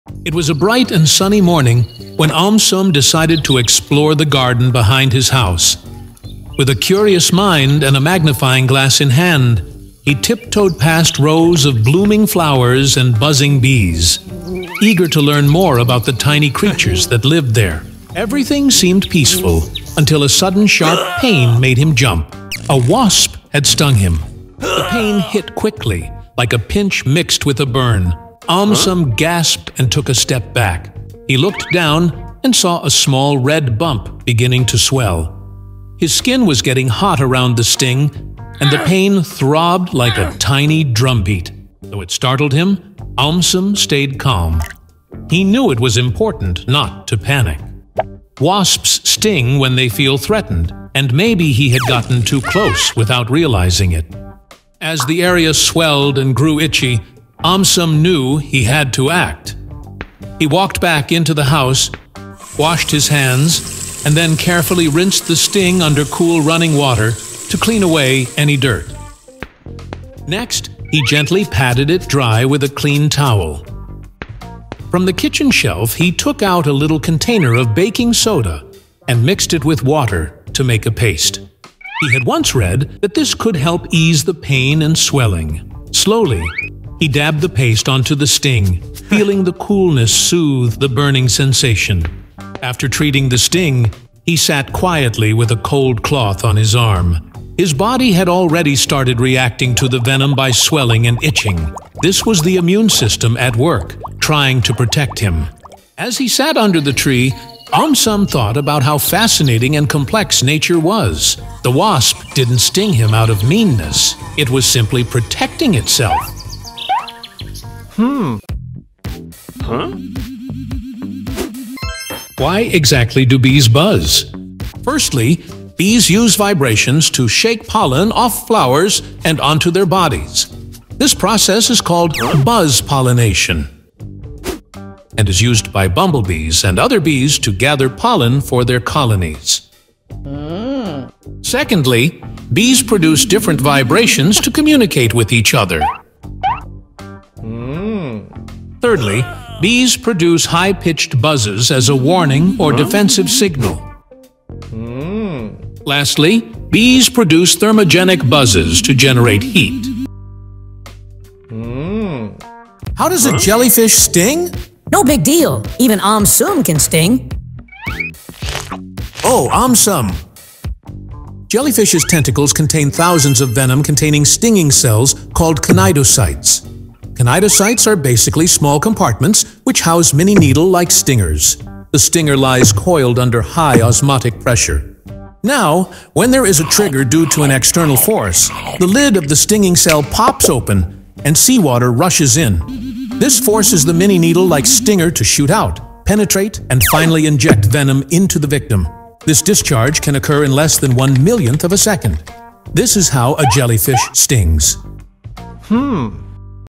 What if a Wasp Stings You? + more videos
It was a bright and sunny morning when AumSum decided to explore the garden behind his house. With a curious mind and a magnifying glass in hand, he tiptoed past rows of blooming flowers and buzzing bees, eager to learn more about the tiny creatures that lived there.

Everything seemed peaceful until a sudden sharp pain made him jump. A wasp had stung him.

The pain hit quickly, like a pinch mixed with a burn. AumSum gasped and took a step back. He looked down and saw a small red bump beginning to swell. His skin was getting hot around the sting, and the pain throbbed like a tiny drumbeat.

Though it startled him, AumSum stayed calm. He knew it was important not to panic. Wasps sting when they feel threatened, and maybe he had gotten too close without realizing it.

As the area swelled and grew itchy, AumSum knew he had to act. He walked back into the house, washed his hands, and then carefully rinsed the sting under cool running water to clean away any dirt.

Next, he gently patted it dry with a clean towel. From the kitchen shelf, he took out a little container of baking soda and mixed it with water to make a paste.

He had once read that this could help ease the pain and swelling. Slowly, he dabbed the paste onto the sting, feeling the coolness soothe the burning sensation.

After treating the sting, he sat quietly with a cold cloth on his arm. His body had already started reacting to the venom by swelling and itching. This was the immune system at work, trying to protect him.

As he sat under the tree, AumSum thought about how fascinating and complex nature was. The wasp didn’t sting him out of meanness. It was simply protecting itself.